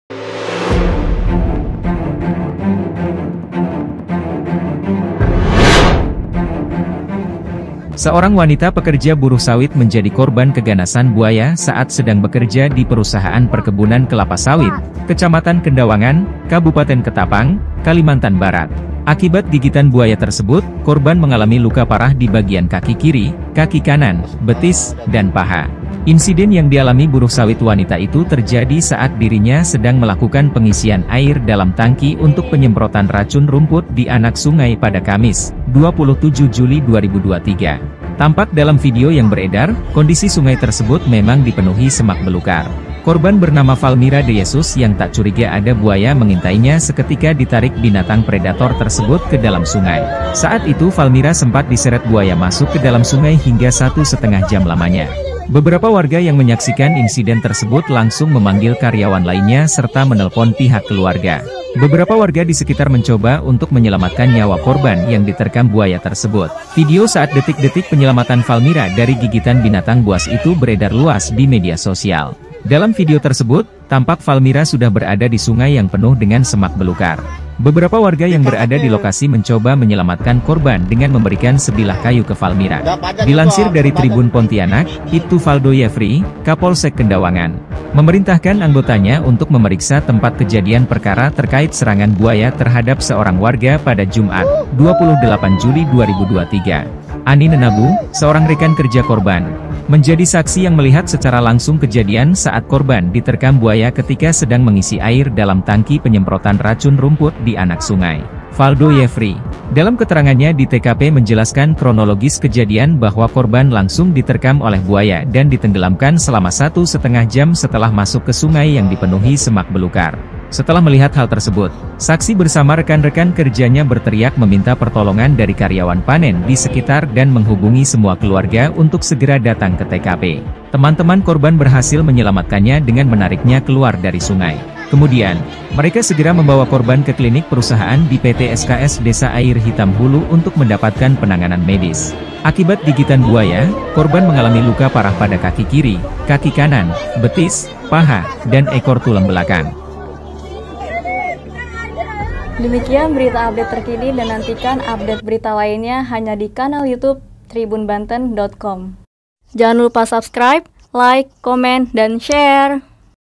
Buruh Wanita Diterkam Buaya Saat Bekerja di Kebun Sawit, Diseret ke Sungai hingga Alami Luka Parah
Seorang wanita pekerja buruh sawit menjadi korban keganasan buaya saat sedang bekerja di perusahaan perkebunan kelapa sawit, Kecamatan Kendawangan, Kabupaten Ketapang, Kalimantan Barat.

Akibat gigitan buaya tersebut, korban mengalami luka parah di bagian kaki kiri, kaki kanan, betis, dan paha.

Insiden yang dialami buruh sawit wanita itu terjadi saat dirinya sedang melakukan pengisian air dalam tangki untuk penyemprotan racun rumput di anak sungai pada Kamis (27/7/2023).

Tampak dalam video yang beredar, kondisi sungai tersebut memang dipenuhi semak belukar.

Korban bernama Falmira de Yesus yang tak curiga ada buaya mengintainya seketika ditarik binatang predator tersebut ke dalam sungai.

Saat itu Falmira sempat diseret buaya masuk ke dalam sungai hingga satu setengah jam lamanya.

Beberapa warga yang menyaksikan insiden tersebut langsung memanggil karyawan lainnya serta menelpon pihak keluarga.

Beberapa warga di sekitar mencoba untuk menyelamatkan nyawa korban yang diterkam buaya tersebut.

Dalam video tersebut, tampak Falmira sudah berada di sungai yang penuh dengan semak belukar. Beberapa warga yang berada di lokasi mencoba menyelamatkan korban dengan memberikan sebilah kayu ke Falmira.